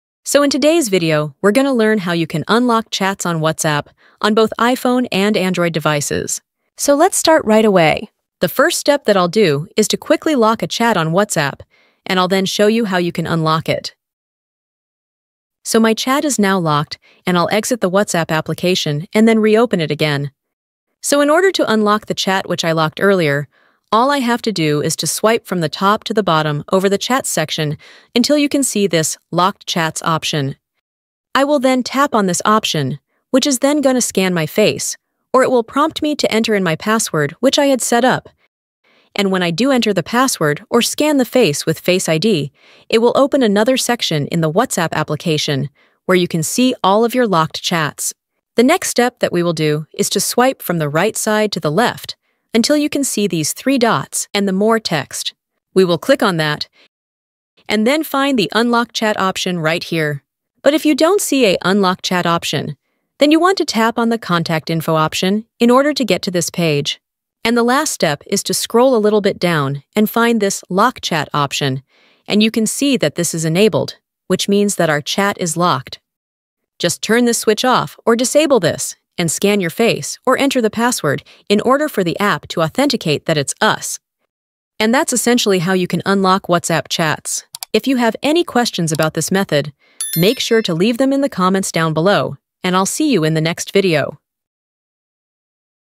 How To Unlock WhatsApp Chats
Unlocking your WhatsApp chats is easier than you think! 🚀 In this video, I'll guide you step-by-step on how to unlock your WhatsApp chats and access your messages with ease.